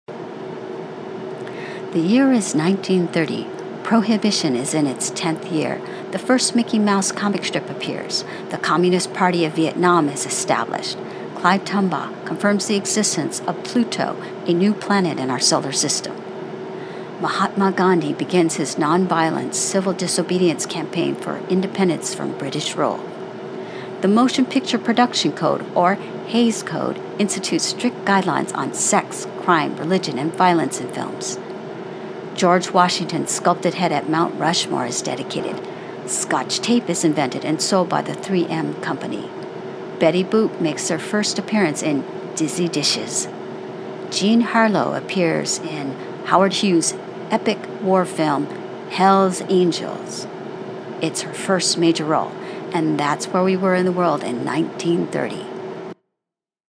The Year Was 1930
To get people in the right mindset for BLUES FOR AN ALABAMA SKY, I thought this short video would help.